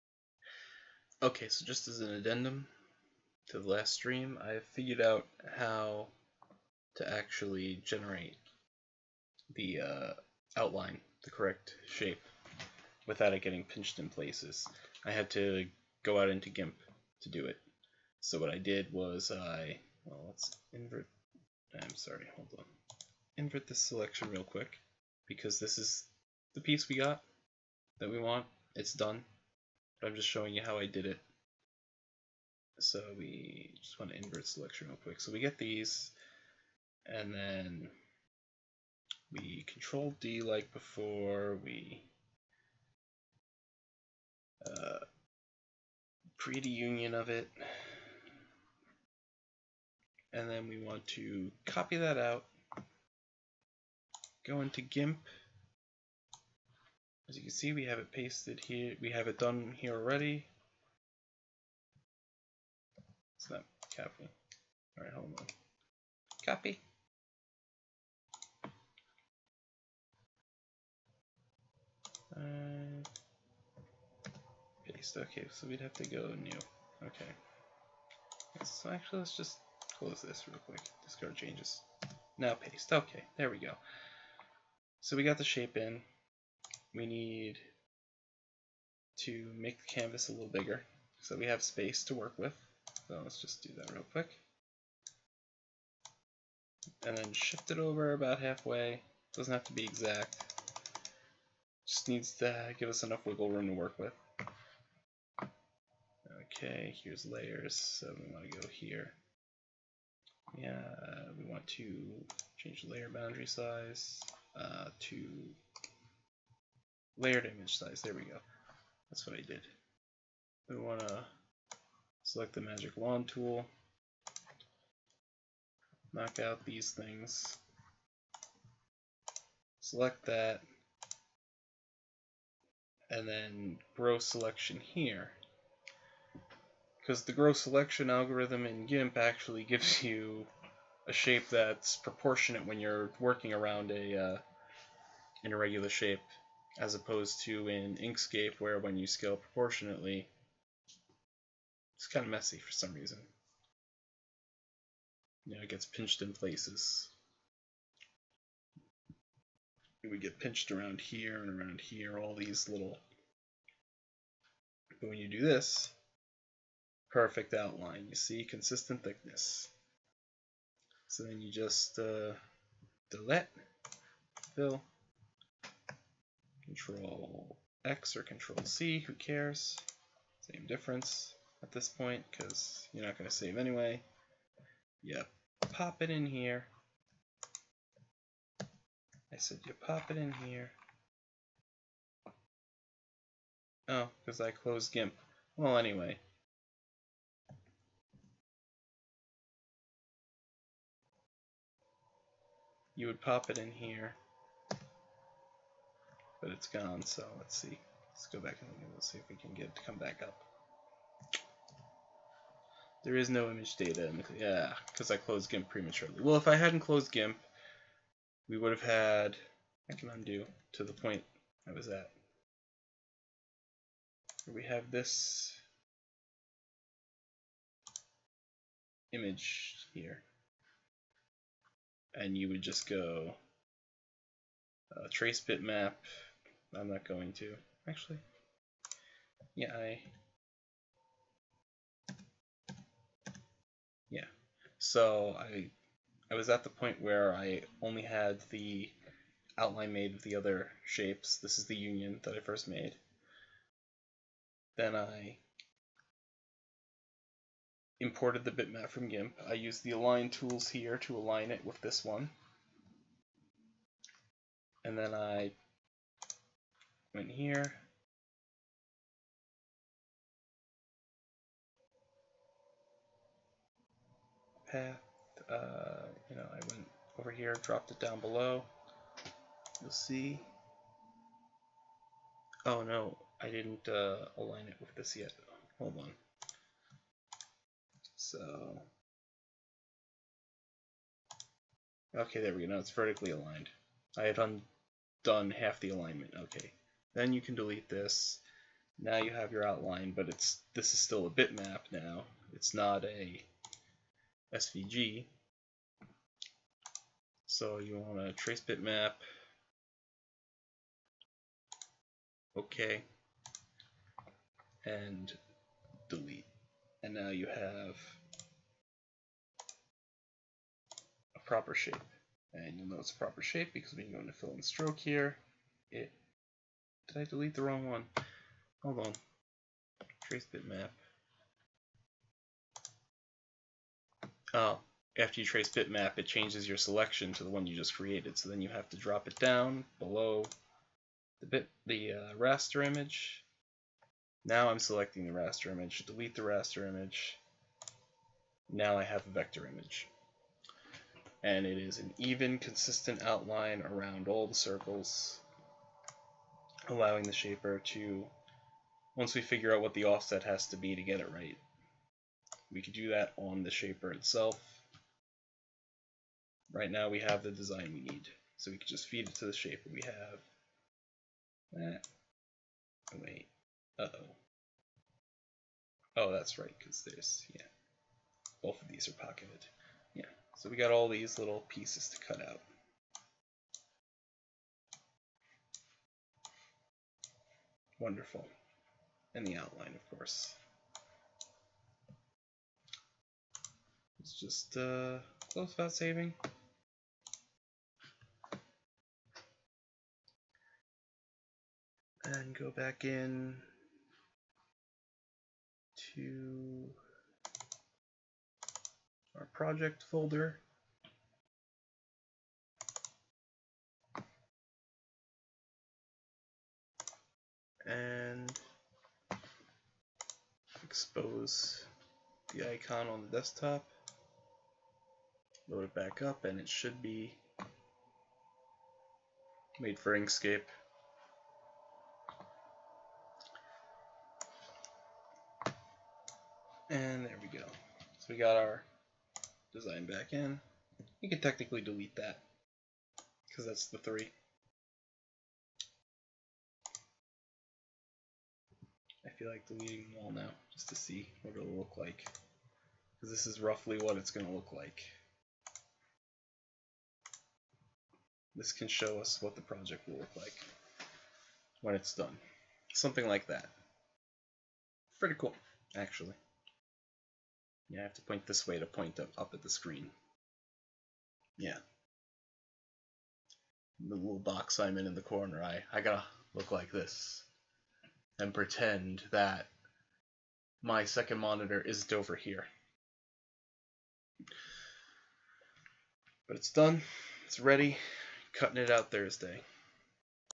Stream 1 Addendum
Creating an even and consistent outline around a collection of shapes in Inkscape requires some creative use of GIMP and Grow Selection